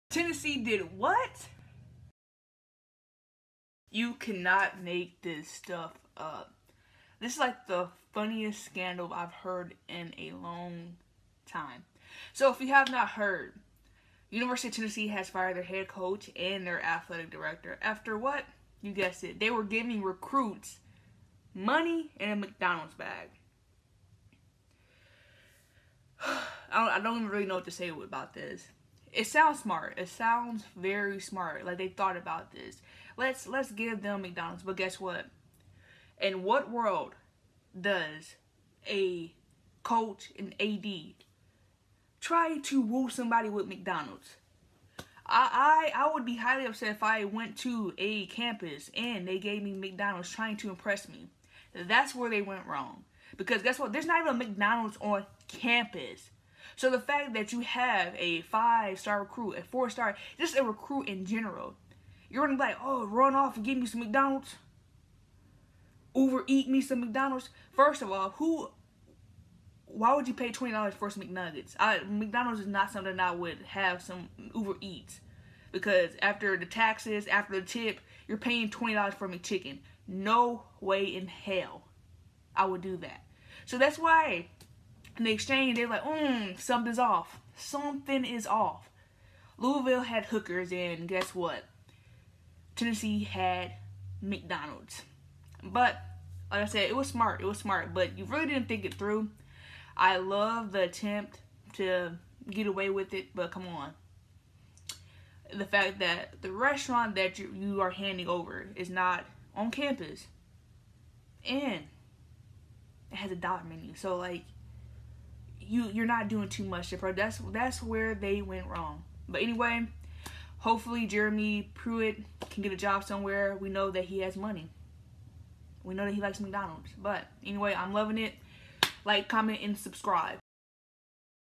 Tennessee DID WHAT? McDonald's recruiting SCANDAL
What went wrong during Tennessee's football money exchange? Not enough hundreds?